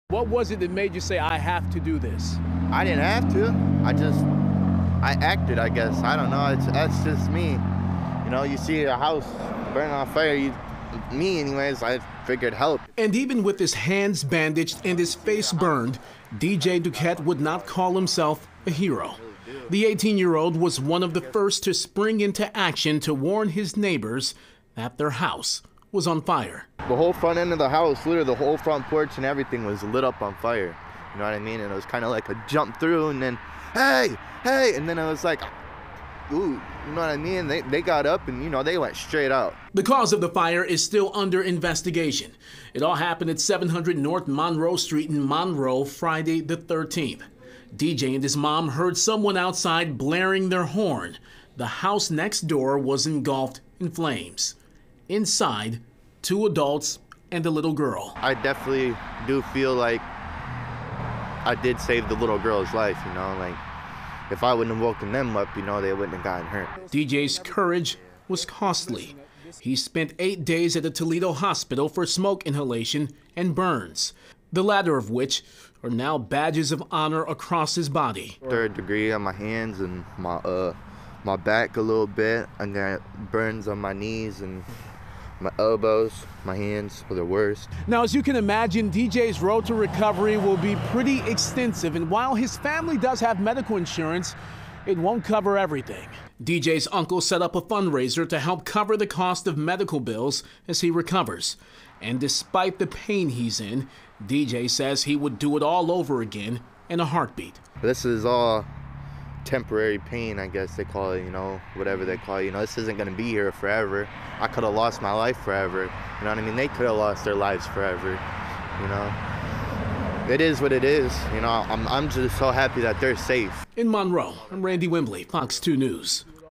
Monroe man saves family from burning home
A Monroe man is recovering after he ran into flames to rescue his neighbors from their burning house. DJ Duquette saved two adults and a girl who were inside the home.